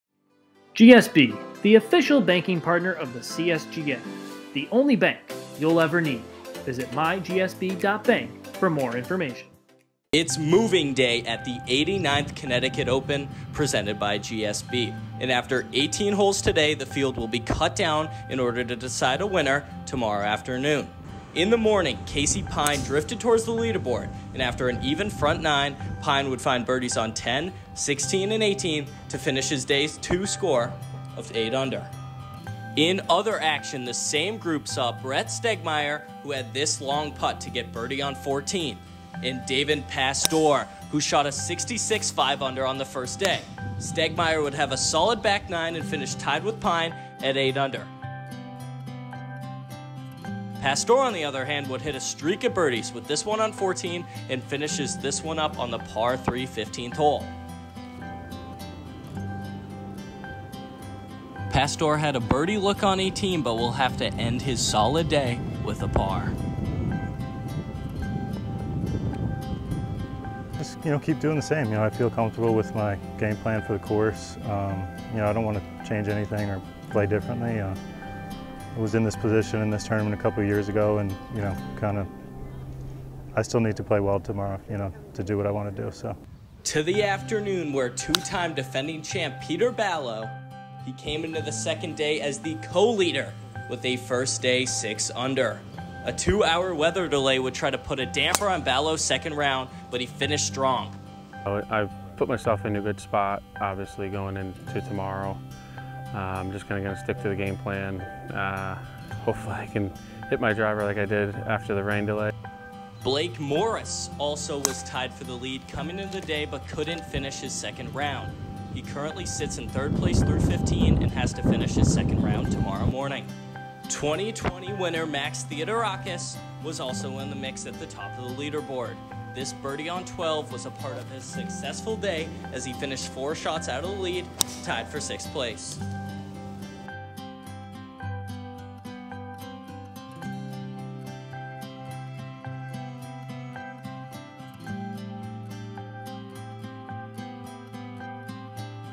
Peter Ballo, David Pastore Share Lead at Suspended 89th Connecticut Open
Peter Ballo will have a chance to three-peat but he will be challenged by co-leader David Pastore and a number of other contenders in hot pursuit. Round two of the 89th Connecticut Open presented by GSB hosted by Shuttle Meadow Country Club was suspended due to darkness.